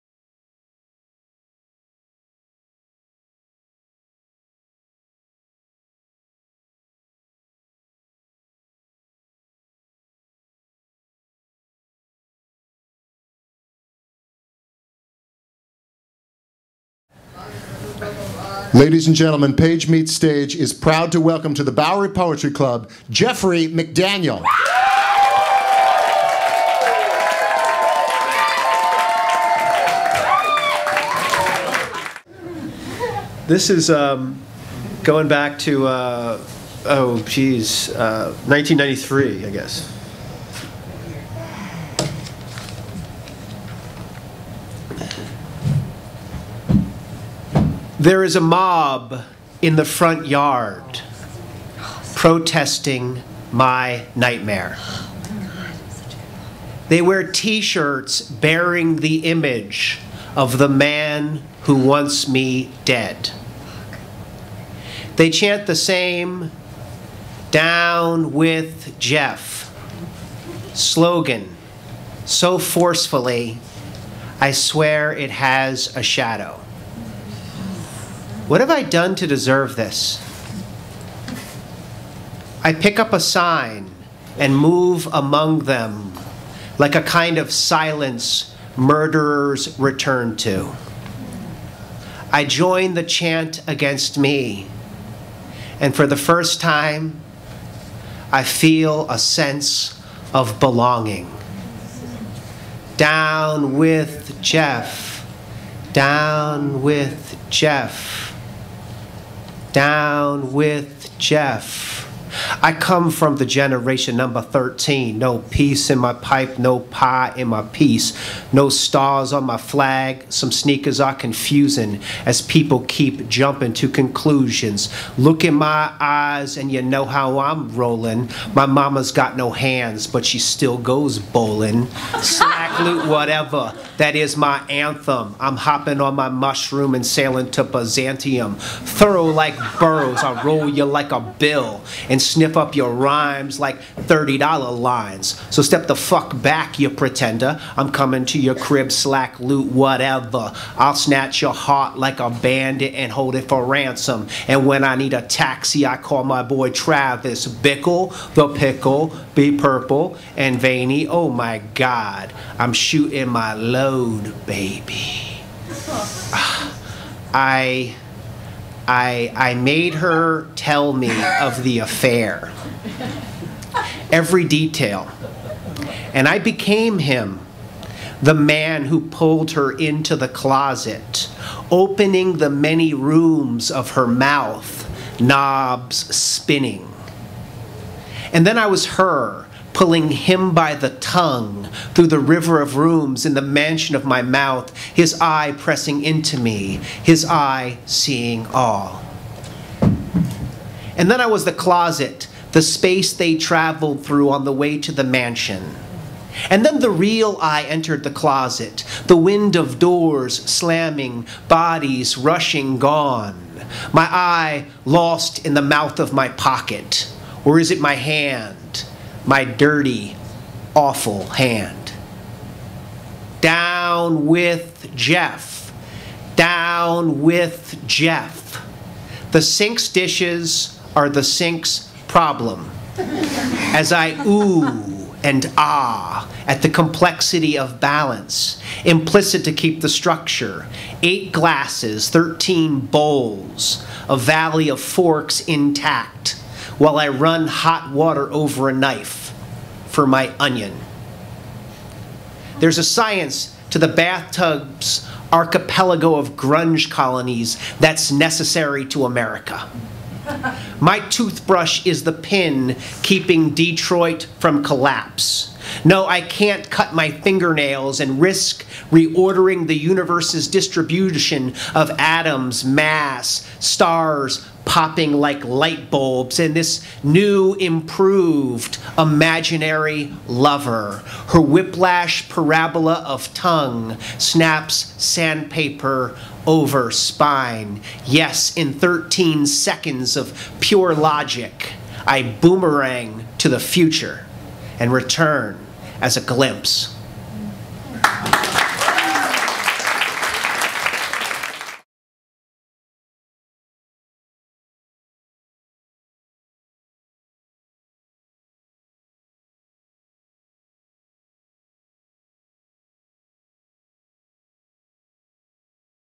Jeffrey McDaniel's "Down with Jeff"
Jeffrey McDaniel, as part of Taylor & Marie Elizabeth's Mali's Page Meets Stage series at the Bowery Poetry Club performs "Down with Jeff". Jeffrey McDaniel is the author of three books: Alibi School (Manic D, 1995), The Forgiveness Parade (Manic D, 1998), and The Splinter Factory (Manic D, 2002). His poems have appeared in numerous journals and anthologies, including Best American Poetry 1994 and New (American) Poets. He is a recipient of an NEA Fellowship, He lives in Brooklyn and teaches creative writing at Sarah Lawrence College.